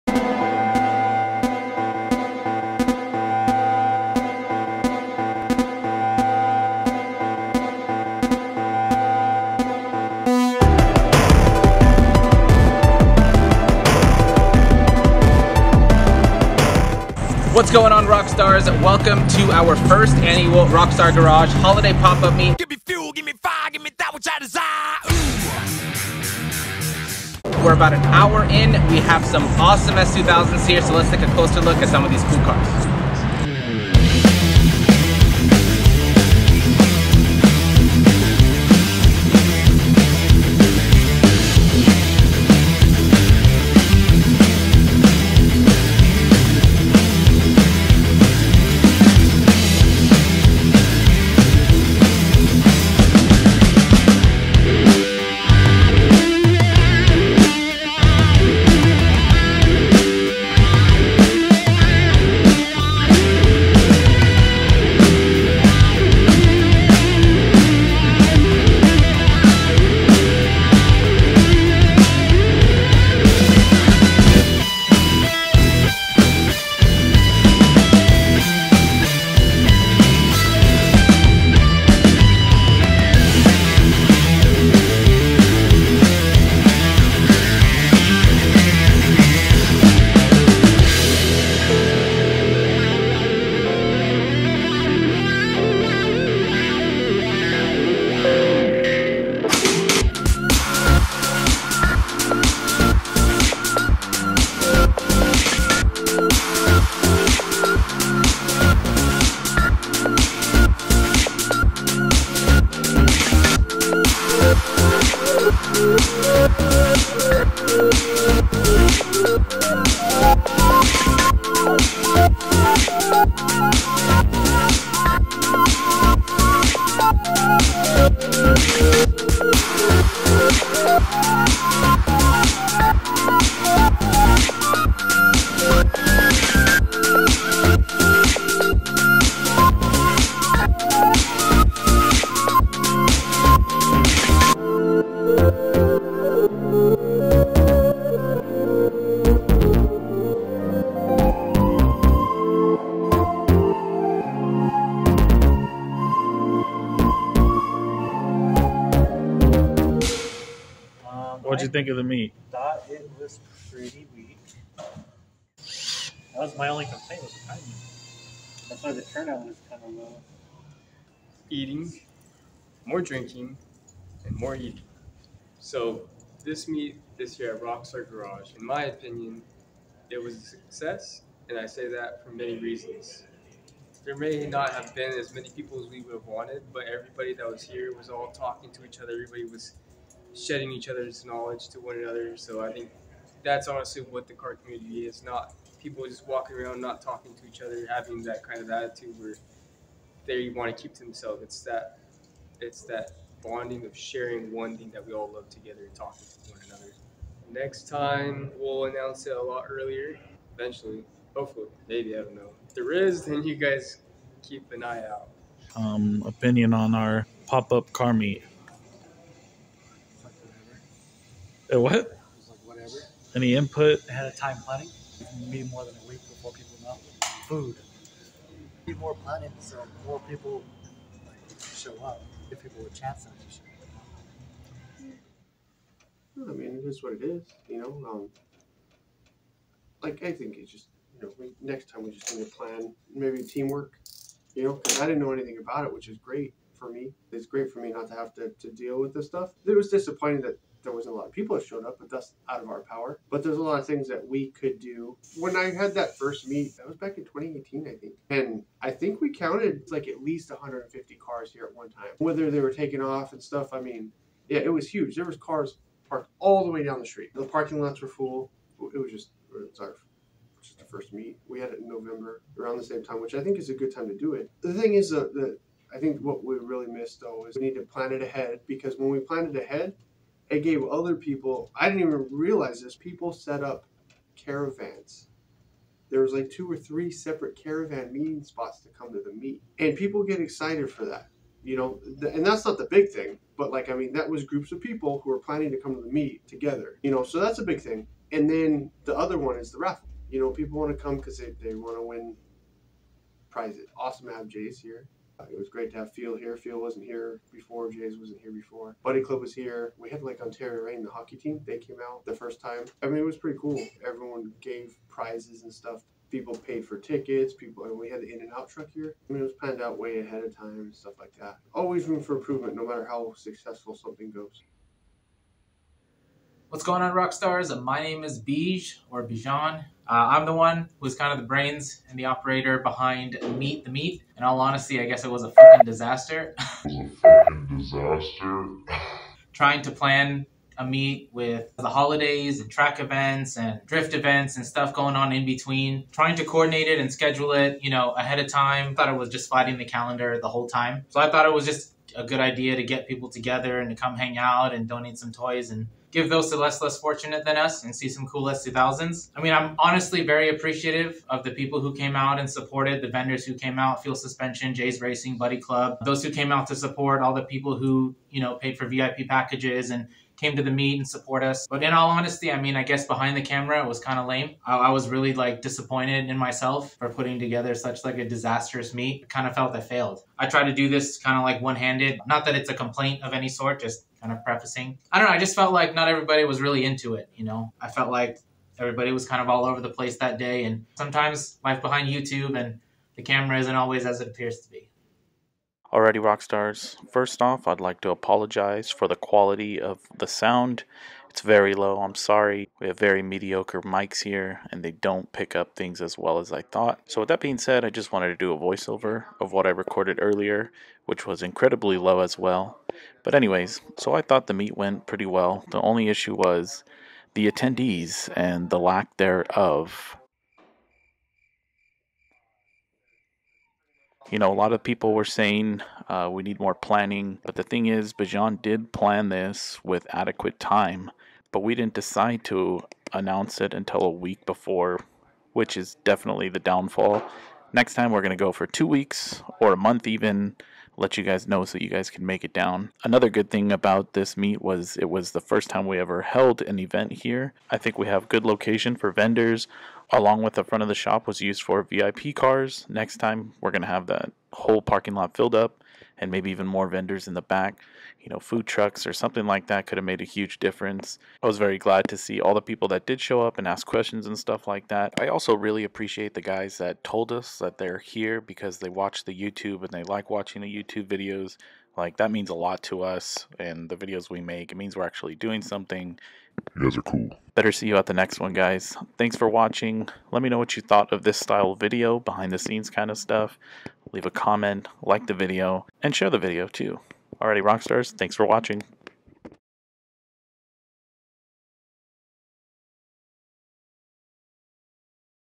Rockstar Garages' First Annual Holiday Pop-Up Meet
First off thank you to all the attendees & our vendors buddyclub jsracing fealsuspension we hope to see you guys at the next event!
Also Rockstars we are trying something a little different here with sharing the opinions of the guys, no takes or practicing lines just straight raw footage. Hope to see you guys at the next meet!

Again sorry about the audio quality, still working on it.